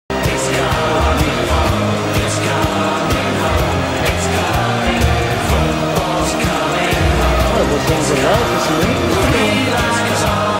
Ronaldinho and Kerry footballer Paul Walsh have somehow ended up doing keepy-uppies down in Rio 😂🇧🇷⚽
Kerry footballer Paul Walsh has somehow ended up doing keepy-uppies with Ronaldinho down in Rio De Janeiro 😂